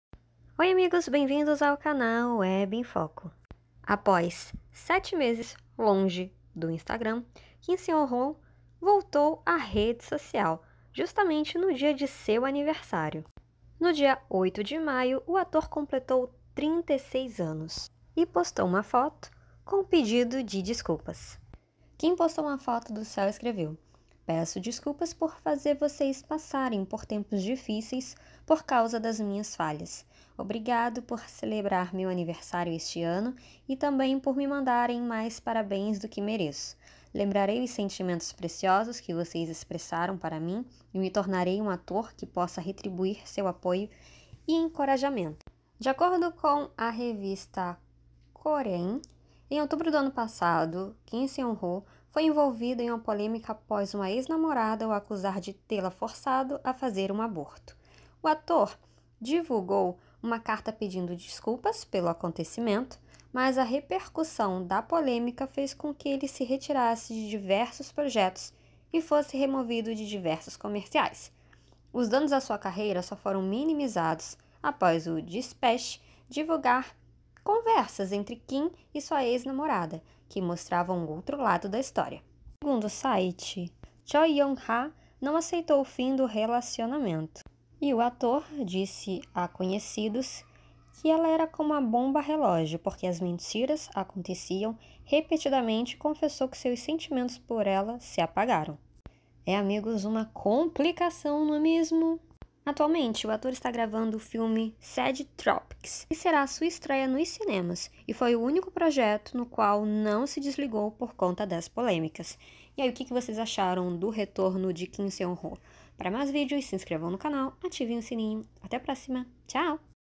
KIM SEON-HO VOLTA AS REDES SOCIAIS APÓS POLEMICA COM EX-NAMORADA
No último domingo (8), o ator Mim Seon-ho retornou ao Instagram após 7 meses longe da rede social após polêmica com sua ex-namorada. O retorno foi justamente no dia de seu aniversário.